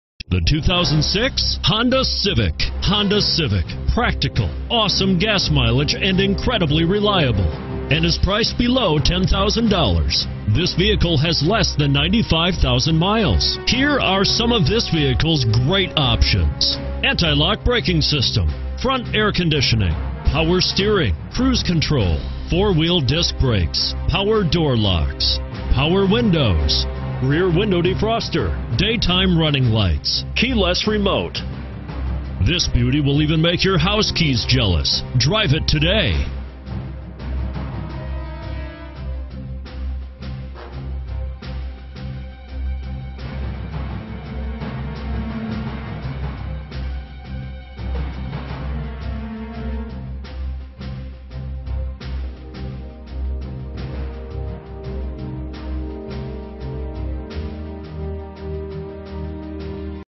2006 Honda Civic Cpe Santa Clara CA GC307331A
2006 Honda Civic Cpe  - Stock#: GC307331A - VIN#: 2HGFG12896H565125

4855 Stevens Creek Blvd.

Check out this 2006 Honda Civic Cpe.   New Arrival! CarFax One Owner! Sunroof/Moonroof, Rear Spoiler, Popular color combo! This Honda Civic gets great fuel economy with over 40.0 MPG on the highway! ABS Brakes Front Wheel Drive Please let us help you with finding the ideal New, Used, or Certified vehicle. Contact the dealership today.